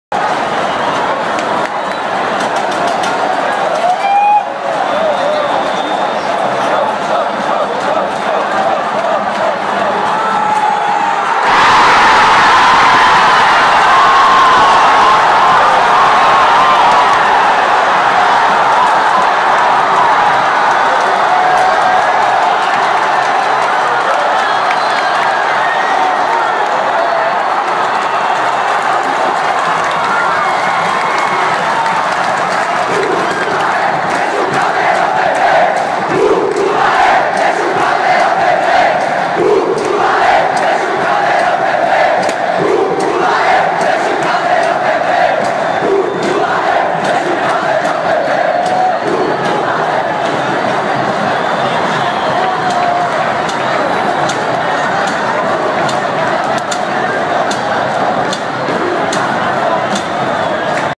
Primeiro gol do Paysandu contra o São Fancisco no dia do Centenário.
Primeiro gol do Paysandu na goleada de 6 x 0 sobre o São Fransisco pelo campeonato Paraense 2014. Jogo no dia do Centenário do Paysandu dia 02/02/2014.